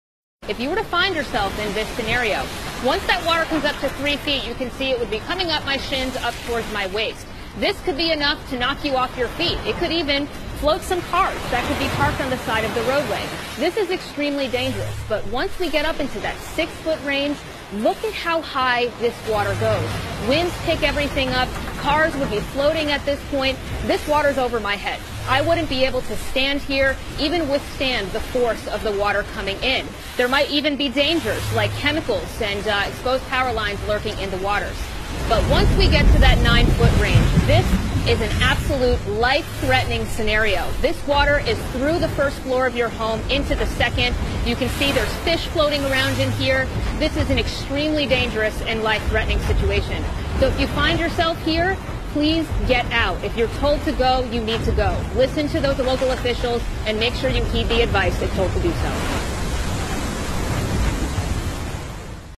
Simulación Huracán Florence
Viernes 14 de Septiembre, 2018.

Increíbles Efectos Especiales | Peculiar forma del WaterChanel de explicar las consecuencias del fenómeno Florence que tocó tierra hace unas horas en las costas del este de EU.

La meteoróloga Erika Navarro, explica con increíbles efectos especiales lo que puede suceder si el nivel del agua va subiendo, hasta llegar a ser un evento que pone en peligro la vida; recomienda acatar las indicaciones de las autoridades y evacuar cuando lo indiquen.

Excelente y realista forma de mostrar los efectos de una inundación.

🎥 Vía @WaterChanel